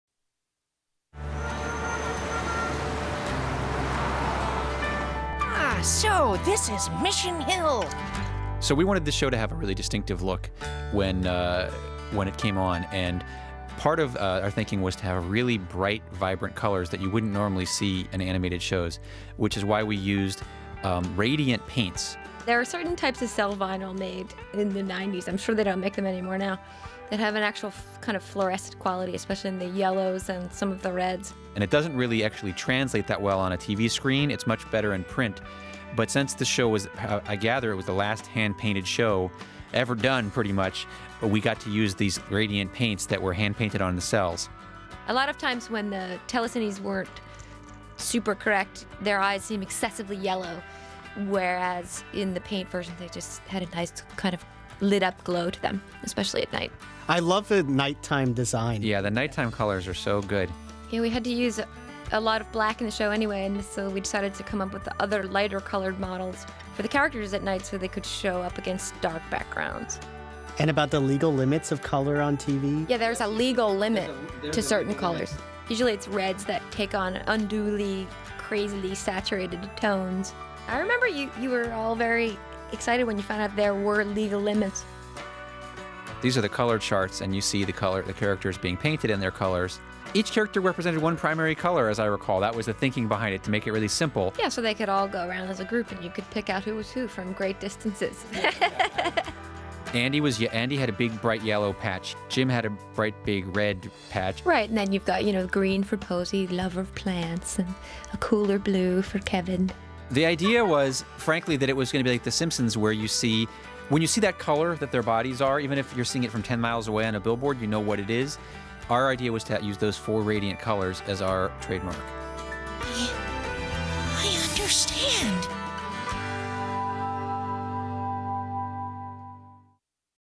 Mission Hill - Radiant Color
Bill Oakley and Lauren MacMullan talk about the color choices for Mission Hill and FCC color limits.

Mission Hill was an animated comedy that ran on the WB for one season, from Fall 1999 to Summer 2000. Created by Bill Oakley and Josh Weinstein fresh off their run at The Simpsons, this cartoon featured the voices of Wallace Langham, Scott Menville, Brian Posehn, Vicki Lewis, Nick Jameson, and Tom Kenny.